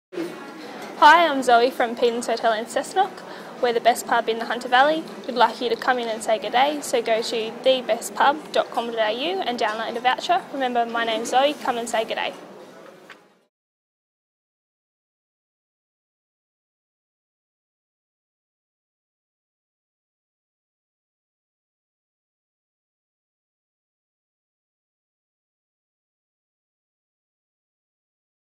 Zoe from Pedens Hotel Cessnock
Zoe from Pedens Hotel Cessnock suggests you come and say "G'day"